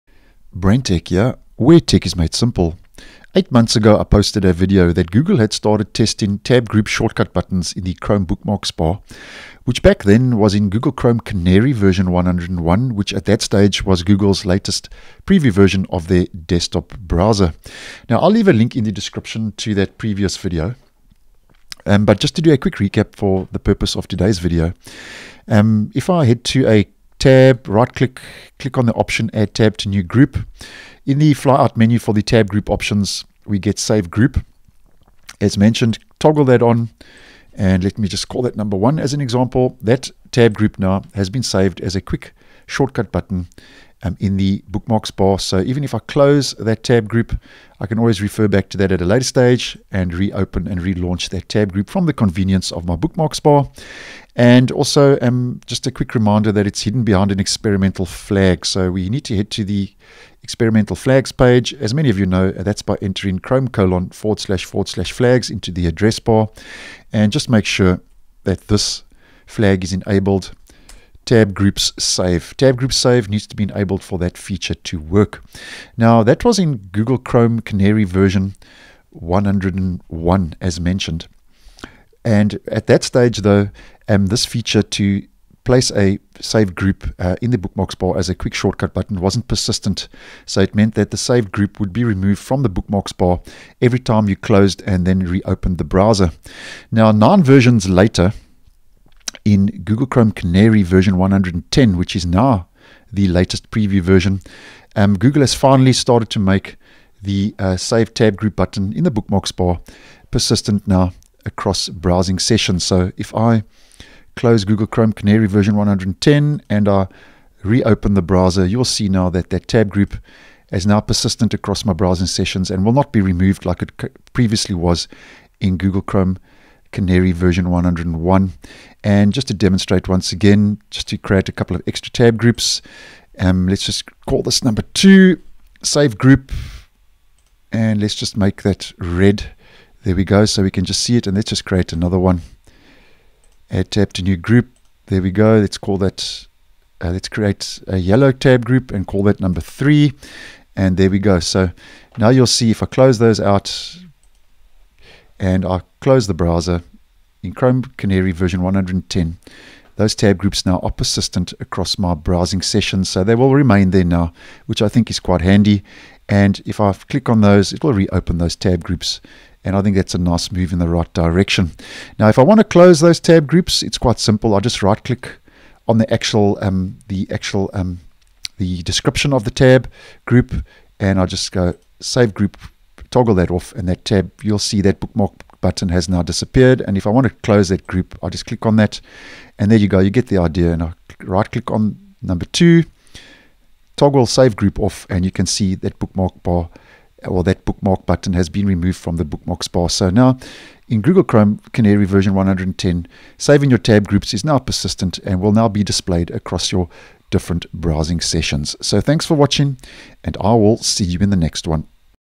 UPDATE Saved Tab groups in Chrome's bookmarks bar are now persistent after a browser restart
In Chrome Canary 101, when you saved a Tab group, you could access your tab group like a normal bookmark from the bookmarks bar Back then, the saving was not persistent, meaning that the saved group would be removed from the bookmarks bar after relaunching the browser. Now as spotted by u/Leopeva64-2 on Reddit, in Chrome Canary 110, Google has finally made the feature persistent across sessions, so that it will not disappear when closing and reopening the browser.